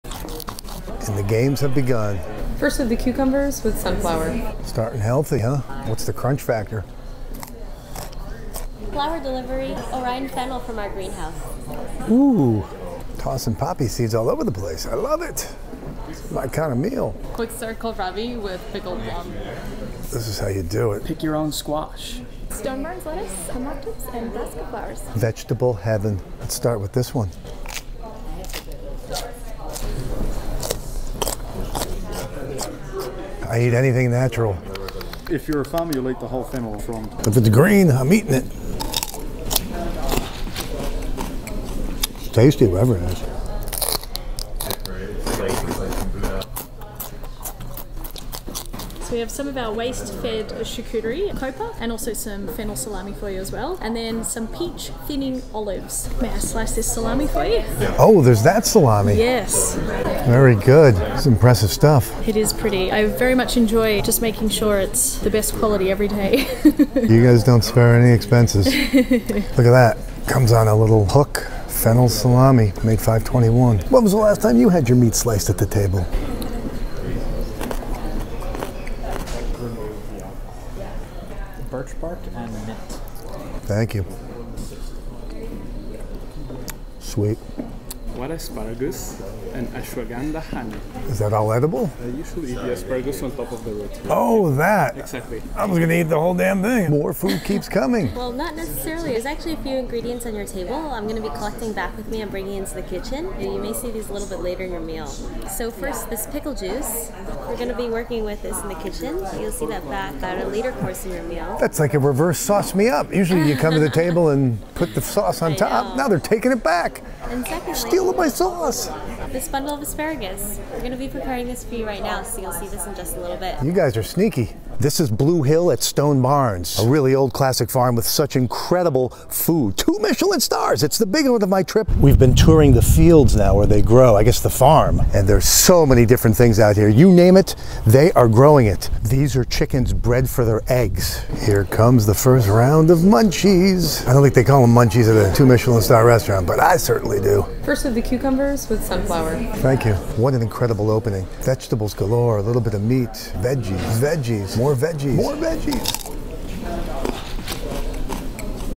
6 Hour 30 Course 2 Michelin Star Meal at Blue Hill at Stone Barns in New York
One of the most incredible meals I've ever had lasted 6 hours and also included a 90 minute tour of their farm.  Creative vegetables and charcuterie started off the 30+ dishes.  This was a heavenly meal.